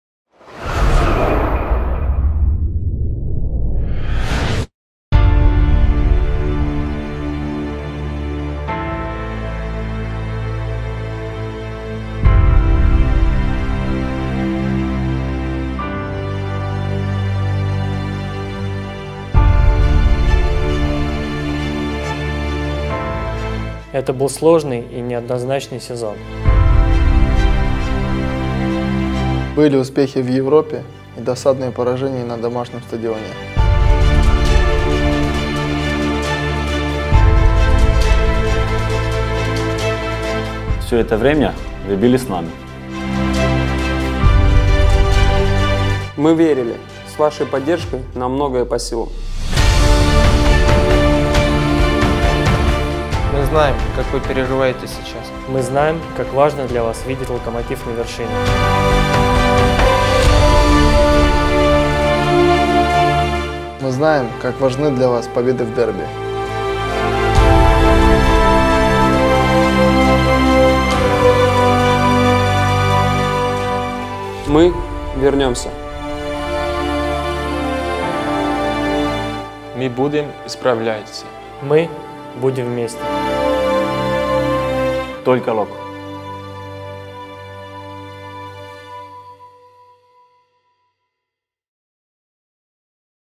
«Мы знаем, как вы переживаете»
Накануне матча с «Мордовией» футболисты «Локомотива» посчитали правильным обратиться к болельщикам с видеообращением. «Мы знаем, как вы переживаете сейчас. Мы переживаем это вместе...»